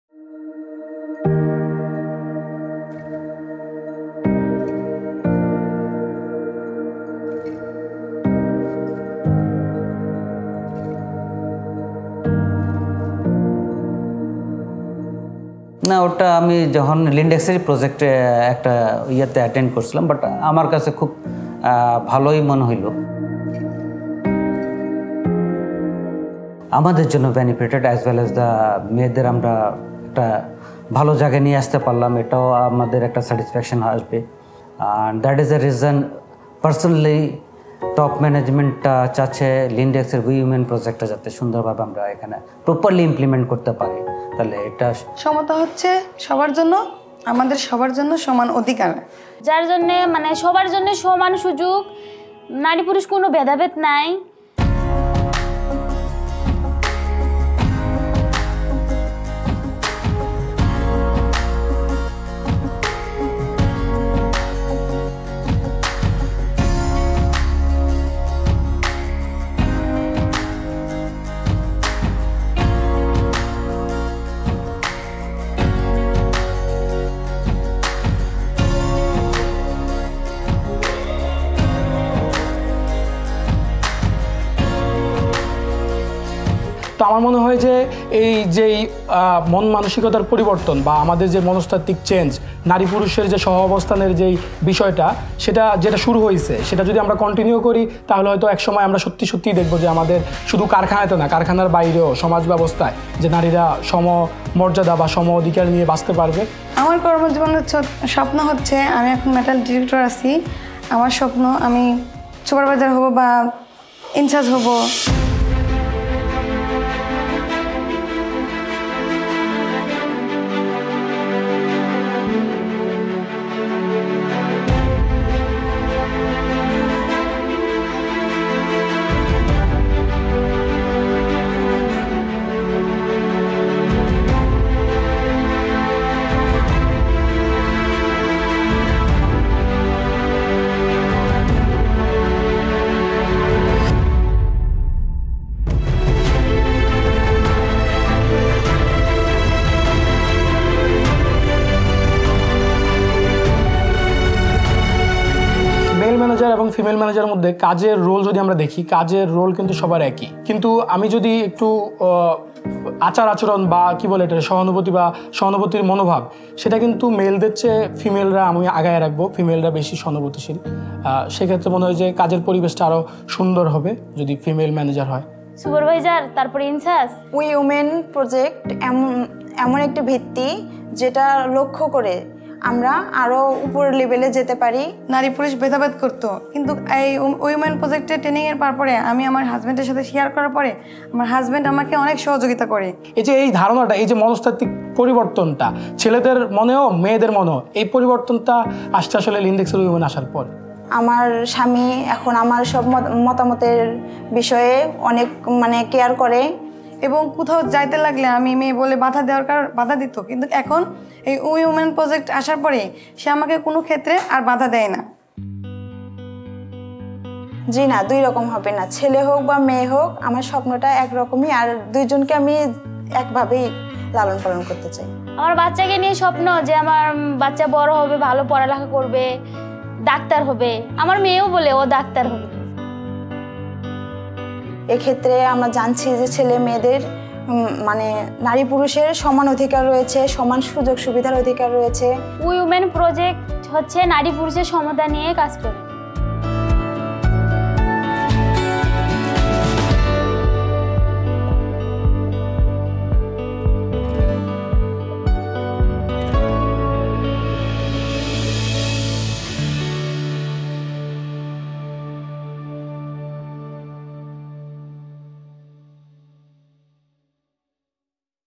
WE Women by Lindex
Learn more about WE Women by Lindex, a programme where we take action for gender equality in our supply chain.